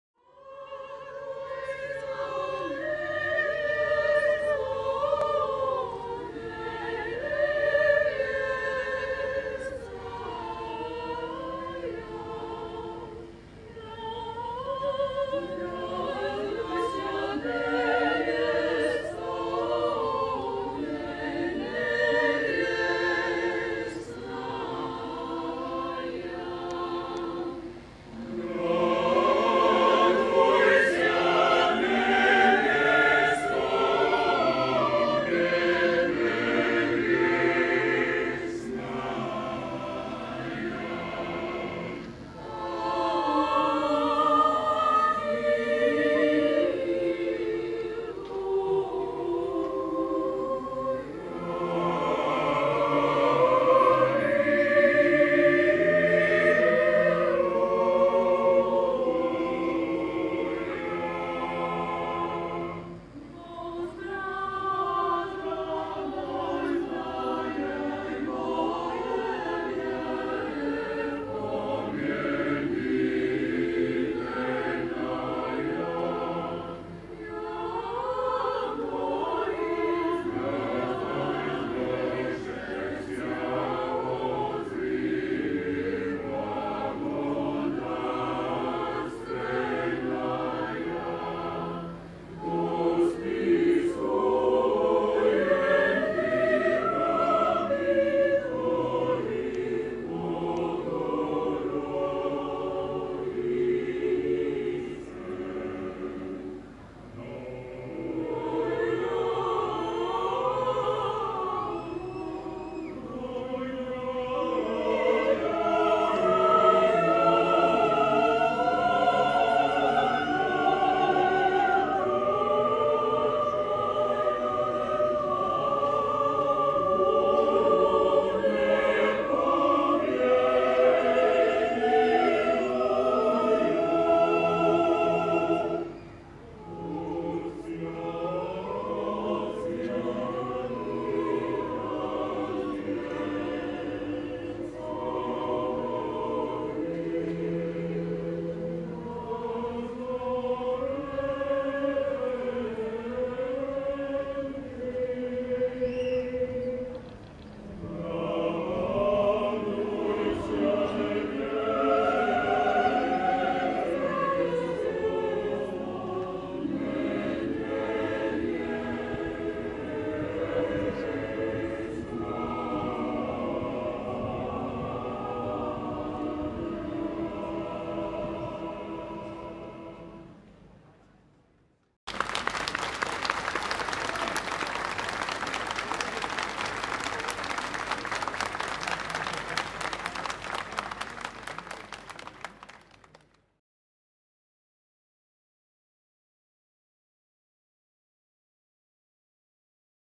Συναυλία Εκκλησιαστικής Μουσικής στον Ι.Ν. Αγίας Φωτεινής στην Παραλία Κατερίνης
Συναυλία της Πολυφωνικής Χορωδίας του Πατριαρχικού Ναού  Βελιγραδίου.
Η Πολυφωνική Χορωδία του Πατριαρχικού Ναού  Βελιγραδίου είναι το παλαιότερο χορωδιακό σύνολο στην Σερβία. Από έναν σύλλογο ψαλτών που το 1853 ίδρυσε μία ομάδα πολιτών, η χορωδία εξελίχθηκε σε  εθνικό θεσμό, που διαφυλάσσει και συνεχίζει την παράδοση που οικοδόμησε κατά τη μακρά περίοδο της ύπαρξής της, συχνά κάτω από δύσκολες συνθήκες. Η ιστορία της, είναι ταυτόχρονα και η ιστορία της εθνικής και πολιτιστικής οικοδόμησης της Σερβίας, όπως και η πνευματική και καλλιτεχνική πηγή της σερβικής σύγχρονης μουσικής.